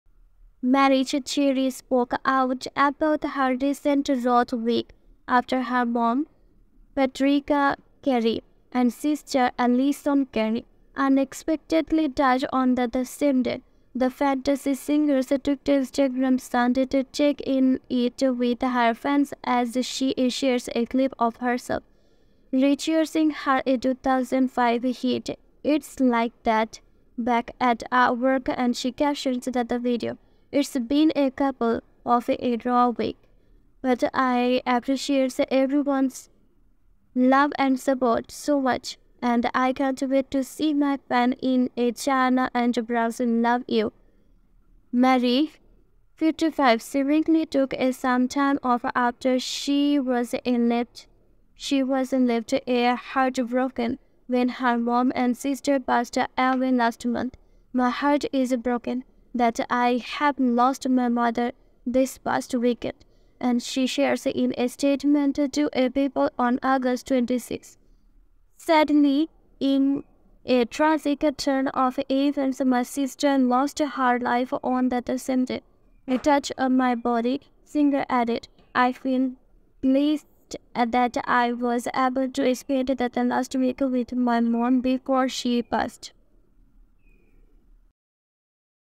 Mariah Carey reflects on a ‘couple of rough weeks’ after her mother and sister’s deaths
**SM BY NEWS ** is your ultimate destination for the latest trends and viral stories